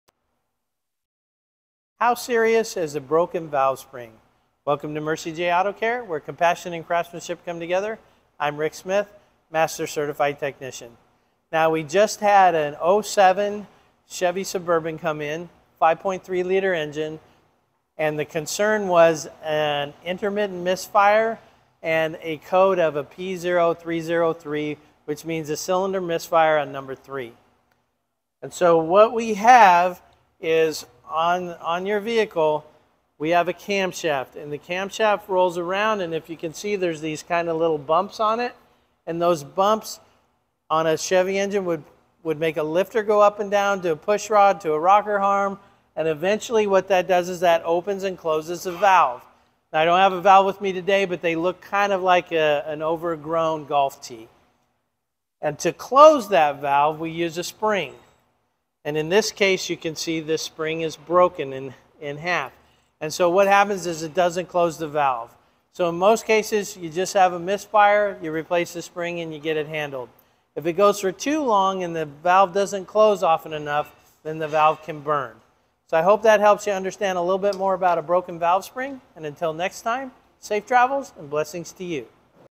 How serious is a Broken Valve Spring?
How serious is a broken valve spring? Welcome to Mercie J Auto Care where compassion and craftsmanship come together. I'm Rick Smith, master certified technician.

We just had an 2007 Chevy Suburban come in, 5.3 L engine and the concern was an intermittent misfire and a code of a P0303, which means a cylinder misfire on number 3. So what we have is on your vehicle, we have a camshaft and the camshaft rolls around and if you can see, there’s these kind a little bumps on it. Those bumps on a Chevy engine what would make a lifter go up and down to push rod to a rocker arm and eventually what that does is that opens and closes a valve. I don’t have a valve with me today, but they look kind of like an overgrown golf tee.

 To close that valve, we use a spring and in this case you can see the spring is broken in half and so what happens is it doesn't close the valve. So in most cases you just have a misfire, you replace the spring and you get it handled.

If it goes for too long and the valve doesn’t close often enough, then the valve can burn. I hope that helps you understand a little bit more about a broken valve spring. Until next time, Safe travels and blessings to you.